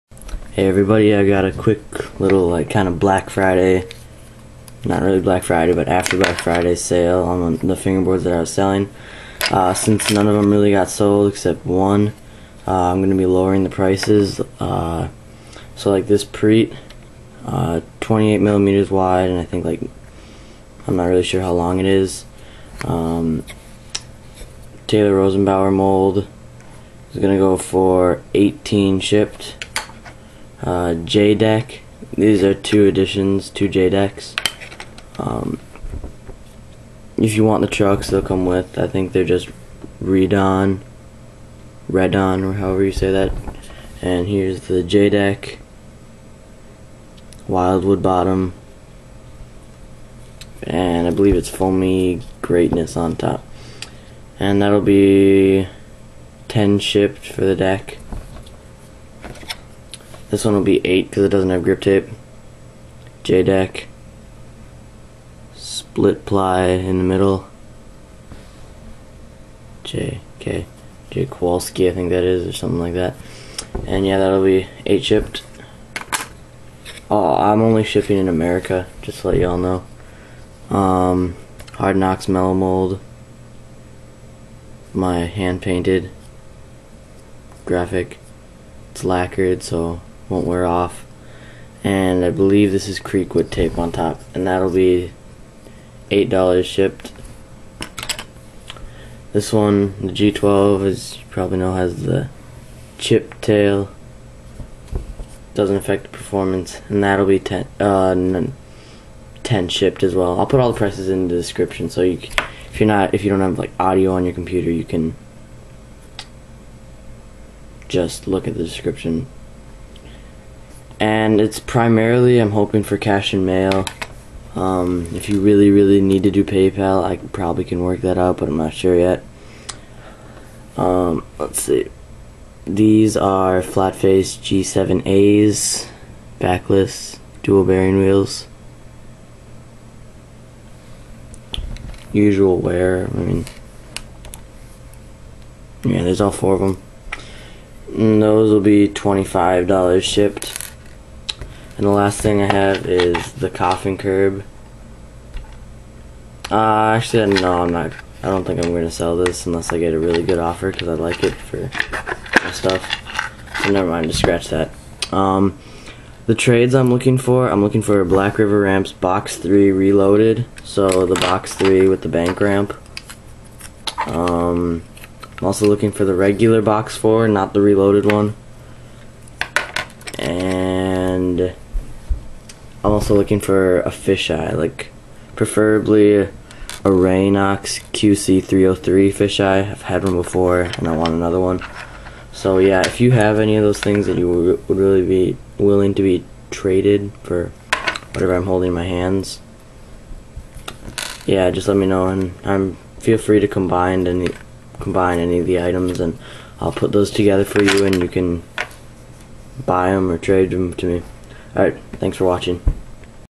Cheaper Fingerboard Sale
prete i'll go $19 shipped
jay decks 10 for the one with tape
8 for the one without tape 17 for both
hardknox mellow fellow 30mm 8 shipped
g7 a's 25 shipped

cash in mail preferred.
the only trades i want are
brr box 3
brr box 3 reloaded
brr kicker
airflo flatbar
but i'm mainly looking for cash